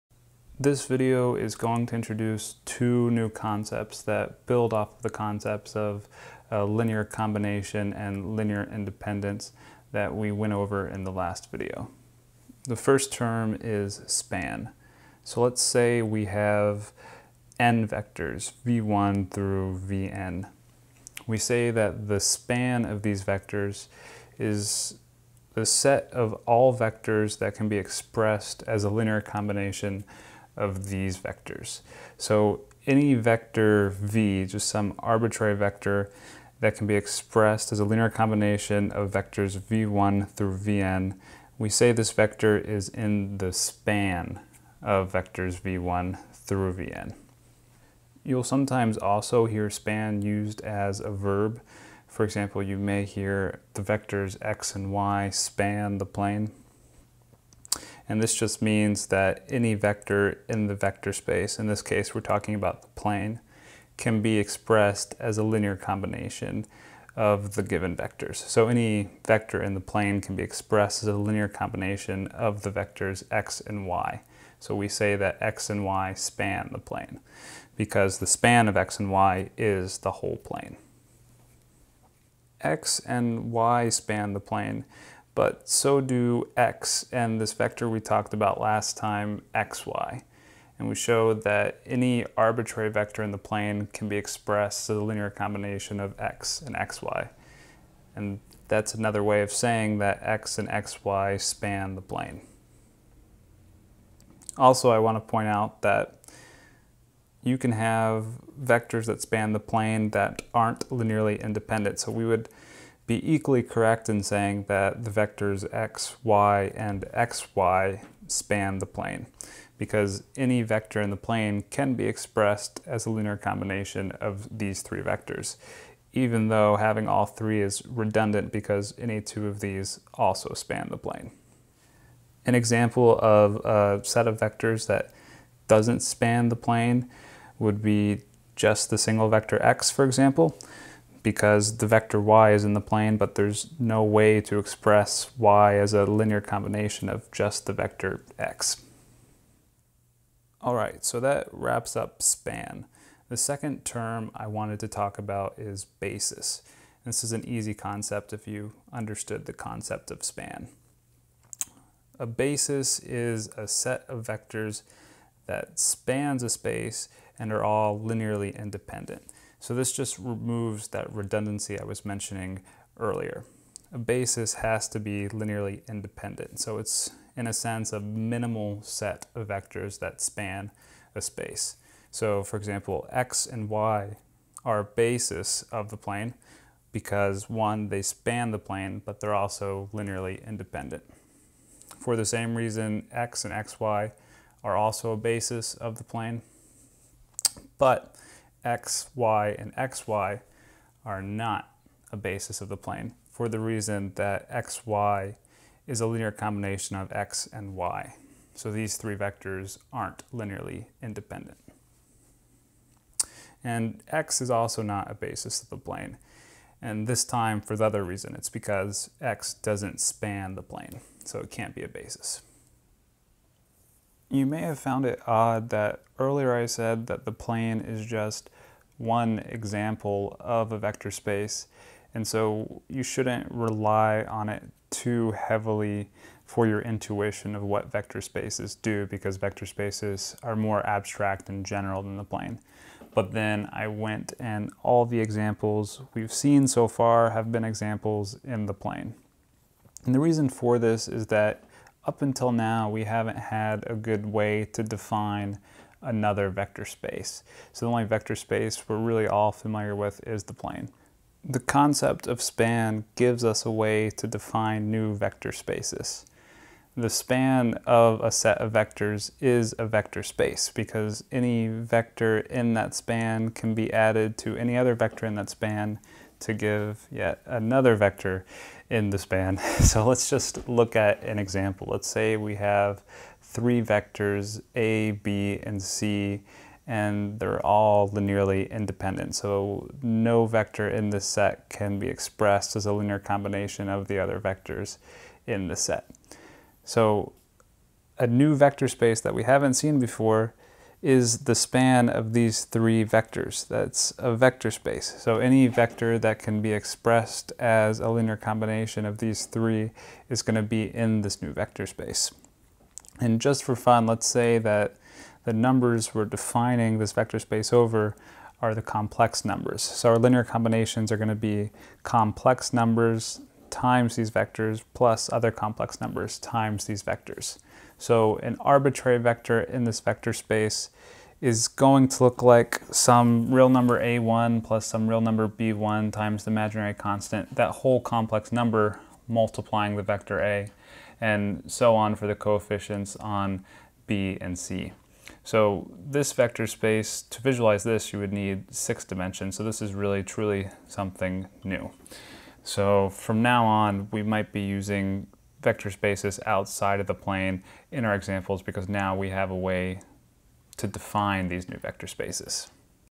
Introduction to Quantum Computing (3) - Span and Basis
The span of a set of vectors is all the vectors that can be expressed as a linear combination of the ones in the set. Span can also be used as a verb. A basis is a set of vectors that both span a space and are linearly independent, so it's a minimal set of vectors that span a space. The span of a set of vectors is a vector space because any vector in the span can be added to any other vector in the span to give another vector in the span, and all the vector space axioms hold. So the concept of span gives us a way to define vector spaces.